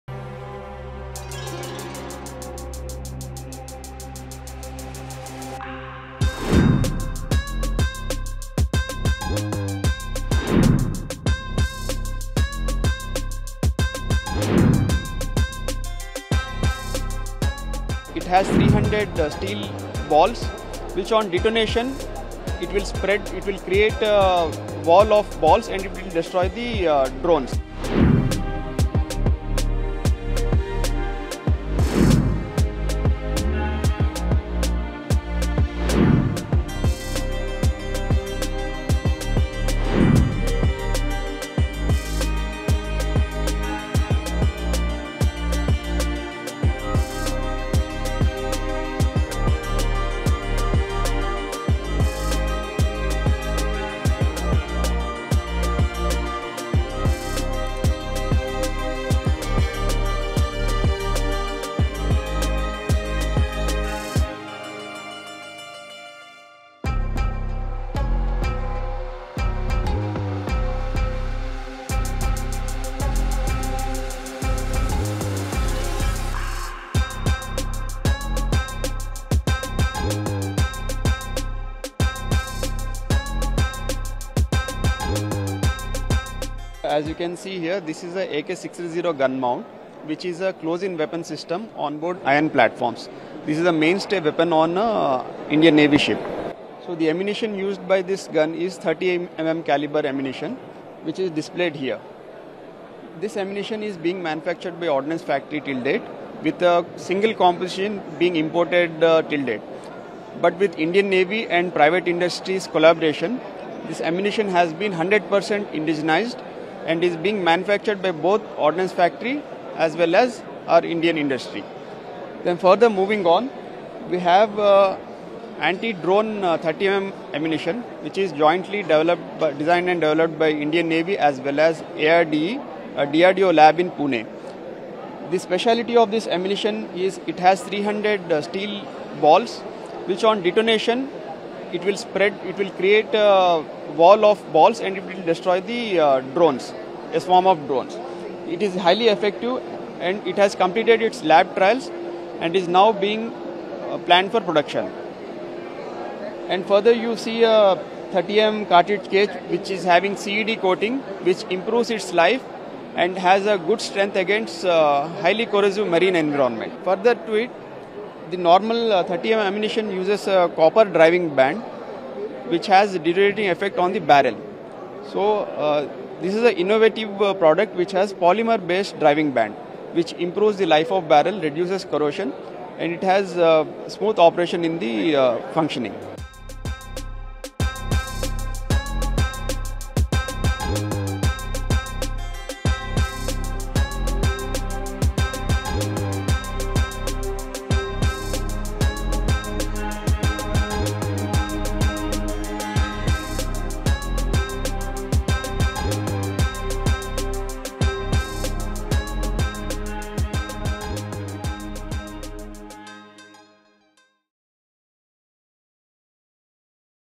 Indian Navy’s 30 mm anti-swarm drone ammunition and ‘Dronaam’ showcased at Swavlamban 2023
The Indian Navy has developed anti-swarm drone 30 mm ammunition. The ammunition was showcased at Indian Navy’s Swavlamban exhibition. This indigenous ammunition system is capable of building an iron wall around warships. This way, it will protect the warships from any attack by enemy swarm drones. At Swavlamban, Dronaam counter-drone system was also showcased. Indian Navy Chief Admiral Hari Kumar held the ‘Dronaam’.